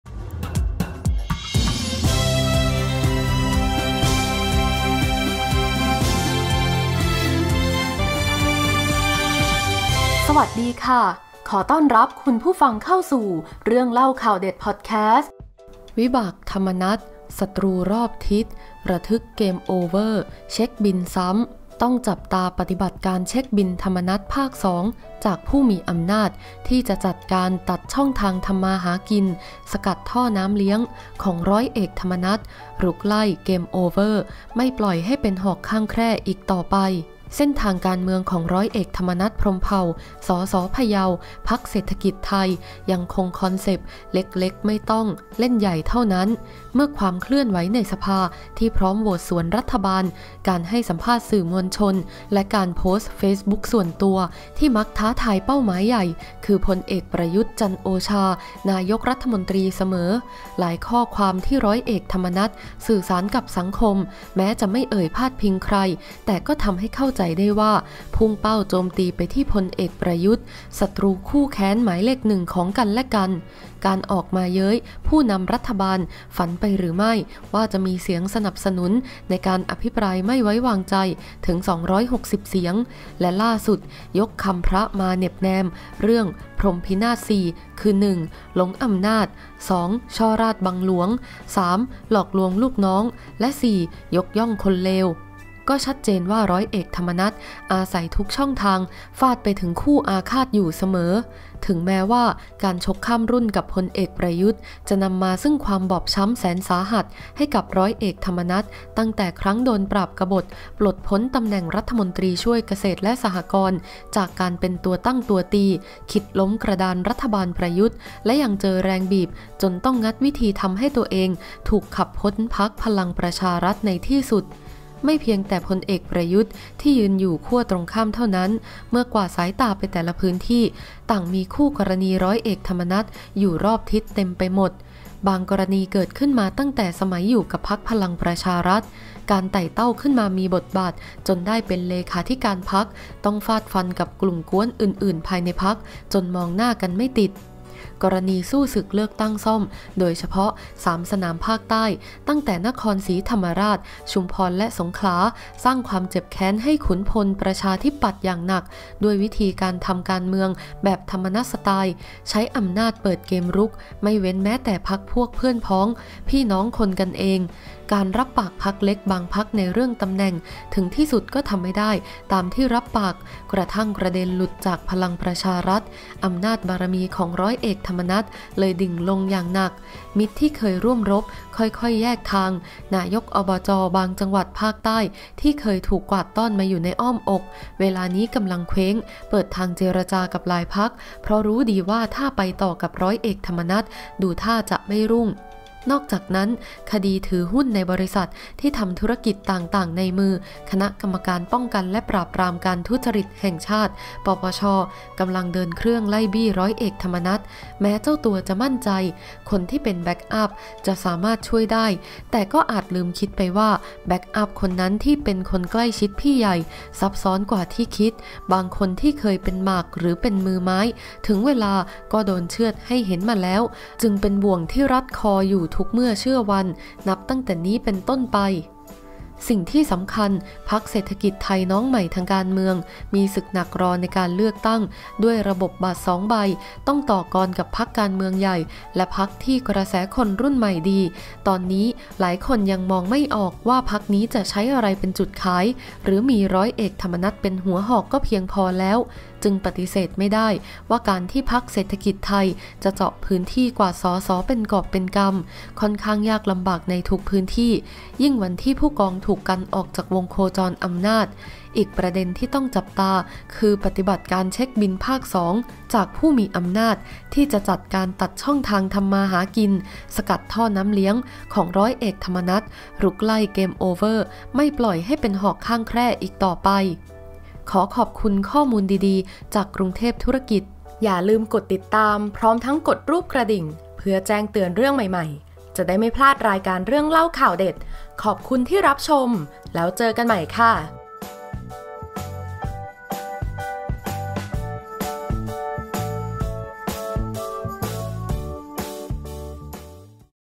วิบาก “ธรรมนัส” ศัตรูรอบทิศ ระทึก เกมโอเวอร์ เช็คบิลซ้ำ
ต้องจับตา ปฏิบัติการเช็คบิล"ธรรมนัส" ภาค 2 จากผู้มีอำนาจ ที่จะจัดการตัดช่องทางทำมาหากิน สกัดท่อน้ำเลี้ยงของ“ร.อ.ธรรมนัส” รุกไล่ เกมโอเวอร์ ไม่ปล่อยให้เป็นหอกข้างแคร่อีกต่อไป

เส้นทางทางการเมืองของ “ร.อ.ธรรมนัส พรหมเผ่า” ส.ส.พะเยา พรรคเศรษฐกิจไทย ยังคงคอนเซ็ปต์ เล็กๆไม่ต้องเล่นใหญ่เท่านั้น เมื่อความเคลื่อนไหวในสภาที่พร้อมโหวตสวนรัฐบาล
การให้สัมภาษณ์สื่อมวลชน และการโพสต์เฟซบุ๊คส่วนตัว ที่มักท้าทายเป้าหมายใหญ่ คือ“พล.อ.ประยุทธ์ จันทร์โอชา” นายกรัฐมนตรี เสมอ

หลายข้อความที่ ร.อ.ธรรมนัส สื่อสารกับสังคม แม้จะไม่เอ่ยพาดพิงถึงใคร แต่ก็ทำให้เข้าใจได้ว่า พุ่งเป้าโจมตีไปที่“พล.อ.ประยุทธ์” ศัตรูคู่แค้นหมายเลข 1 ของกันและกัน

การออกมาเย้ยผู้นำรัฐบาล ฝันไปหรือไม่ ว่าจะมีเสียงสนับสนุนในการอภิปรายไม่ไว้วางใจ ถึง 260 เสียง และล่าสุด ยกคำพระมาเหน็บแนม เรื่อง“พรหมพินาศ 4” คือ
1.หลงอำนาจ
2.ฉ้อราษฎร์บังหลวง
3.หลอกลวงลูกน้อง
และ 4.ยกย่องคนเลว
ก็ชัดเจนว่า ร.อ.ธรรมนัสอาศัยทุกช่องทางฟาดไปถึงคู่อาฆาตอยู่เสมอ

ถึงแม้ว่า การชกข้ามรุ่นกับ“พล.อ.ประยุทธ์” จะนำมาซึ่งความบอบช้ำแสนสาหัสให้กับ“ร.อ.ธรรมนัส” ตั้งแต่ครั้งโดนปราบกบฎ ปลดพ้นตำแหน่ง รมช.เกษตรและสหกรณ์ จากการเป็นตัวตั้งตัวตี
คิดล้มกระดานรัฐบาลประยุทธ์ และยังเจอแรงบีบจนต้องงัดวิธีทำให้ตัวเองถูกขับพ้น“พรรคพลังประชารัฐ”ในที่สุด

ไม่เพียงแต่“พล.อ.ประยุทธ์” ที่ยืนอยู่ขั้วตรงข้ามเท่านั้น เมื่อลองกวาดสายตาไปแต่ละพื้นที่ ต่างก็มีคู่กรณีของ“ร.อ.ธรรมนัส” อยู่รอบทิศทางเต็มไปหมด

บางกรณีเกิดขึ้นมาตั้งแต่สมัยอยู่กับ“พลังประชารัฐ” การไต่เต้าขึ้นมามีบทบาท จนได้เป็นเลขาธิการพรรคฯ ต้องฟาดฟันกับกลุ่มก๊วนอื่นๆภายในพรรค จนมองหน้ากันไม่ติด

กรณีสู้ศึกเลือกตั้งซ่อม โดยเฉพาะ 3 สนามภาคใต้ ตั้งแต่ นครศรีธรรมราช ชุมพร และ สงขลา สร้างความเจ็บแค้นให้ขุนพล“ประชาธิปัตย์” อย่างหนัก ด้วยวิธีการทำการเมืองแบบ“ธรรมนัสสไตล์”
ใช้อำนาจเปิดเกมรุก ไม่เว้นแม้แต่พรรคพวกเพื่อนพ้องพี่น้องคนกันเอง

การรับปากพรรคเล็กบางพรรคในเรื่องตำแหน่ง ถึงที่สุดก็ทำไม่ได้ตามที่รับปาก กระทั่งกระเด็นหลุดจาก“พลังประชารัฐ” อำนาจ บารมี ของ“ร.อ.ธรรมนัส” เลยดิ่งลงอย่างหนัก

มิตรที่เคยร่วมรบค่อยๆแยกทาง นายกฯ อบจ.บางจังหวัดทางภาคใต้ ที่เคยถูกกวาดต้อนมาอยู่ในอ้อมอก เวลานี้กำลังเคว้ง เปิดทางเจรจากับหลายพรรค เพราะรู้ดีว่าถ้าจะไปต่อกับ“ร.อ.ธรรมนัส”
ดูท่าจะไม่รุ่ง

นอกจากนั้น คดีถือหุ้นในบริษัทที่ทำธุรกิจต่างๆ ในมือ“คณะกรรมการป้องกันและปราบปรามการทุจริตแห่งชาติ (ป.ป.ช.)” กำลังเดินเครื่องไล่บี้“ร.อ.ธรรมนัส” แม้เจ้าตัวจะมั่นใจคนที่เป็นแบ็คอัพจะสามารถช่วยได้
แต่ก็อาจลืมคิดไปว่า แบ็คอัพคนนั้นที่เป็นคนใกล้ชิดพี่ใหญ่ ซับซ้อนกว่าที่คิด
บางคนที่เคยเป็นหมาก หรือเป็นมือไม้ ถึงเวลาก็โดนเชือดให้เห็นมาแล้ว ตรงนี้จึงเป็นบ่วงที่รัดคออยู่ทุกเมื่อเชื่อวัน นับแต่นี้เป็นต้นไป

สิ่งสำคัญ “พรรคเศรษฐกิจไทย” น้องใหม่ในทางการเมือง มีศึกหนักรออยู่ในการเลือกตั้ง ด้วยระบบบัตร 2 ใบ ต้องต่อกรกับพรรคการเมืองใหญ่ และพรรคที่กระแสคนรุ่นใหม่ดี
ตอนนี้หลายคนก็ยังมองไม่ออก ว่าพรรคนี้จะใช้อะไรเป็นจุดขาย หรือมี“ร.อ.ธรรมนัส”เป็นหัวหอก ก็เพียงพอแล้ว

จึงปฏิเสธไม่ได้ว่า การที่พรรคเศรษฐกิจไทยจะเจาะพื้นที่กวาด ส.ส.เป็นกอบกำ ค่อนข้างยากลำบากในทุกพื้นที่ ยิ่งในวันที่“ผู้กอง” ถูกกันออกจากวงโคจรอำนาจ

อีกประเด็นที่ต้องจับตา คือ ปฏิบัติการเช็คบิลภาค 2 จากผู้มีอำนาจ ที่จะจัดการตัดช่องทางทำมาหากิน สกัดท่อน้ำเลี้ยงของ“ร.อ.ธรรมนัส” รุกไล่ เกมโอเวอร์ ไม่ปล่อยให้เป็นหอกข้างแคร่อีกต่อไป

ขอขอบคุณข้อมูลดีๆจาก กรุงเทพธุรกิจ